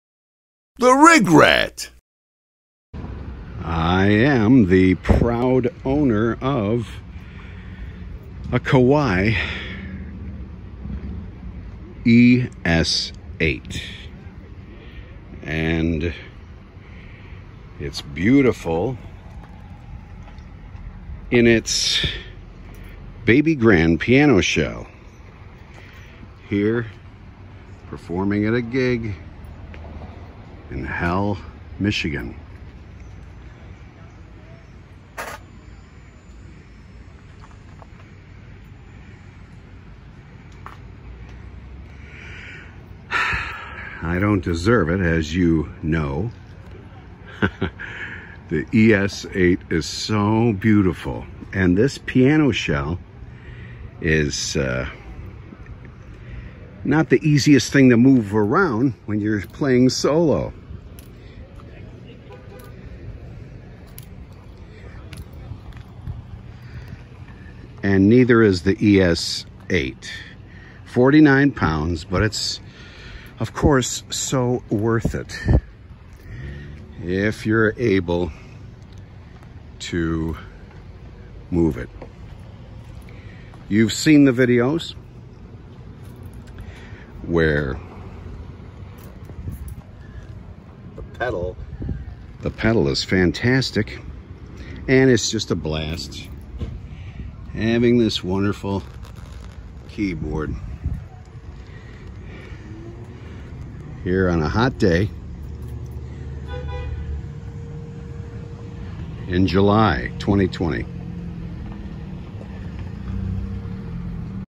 Kawai ES8 in a 4 Foot Baby Grand Piano Shell
Kawai ES8 in a 4 foot baby grand piano shell. It's a material world. Lucky to have this current rig. PlanBPianoShell